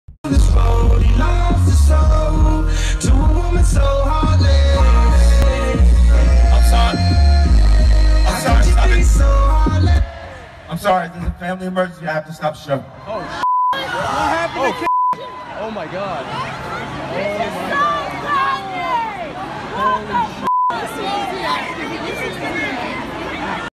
Kanye West forced to stop show early after Kim Kardashian ROBBED at GUNPOINT [VIDEO]
Kanye West forced to stop show early after Kim Kardashian ROBBED at GUNPOINT